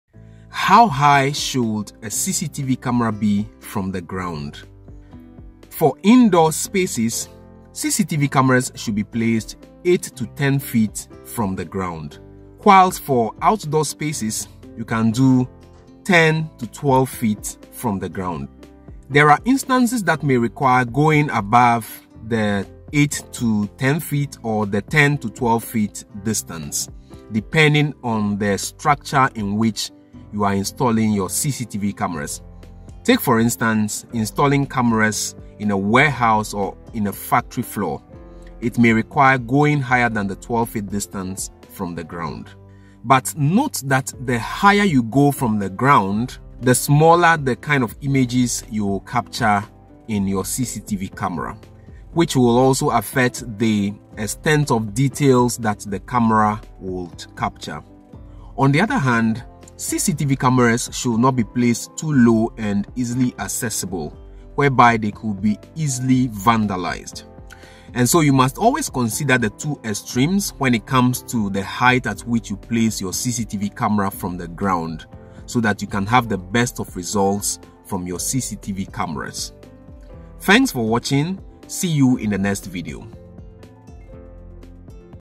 Acceptable height to place cctv cameras
The acceptable height to place cctv cameras for best results and safety.
The acceptable height for indoor spaces
Acceptable height for outdoor spaces.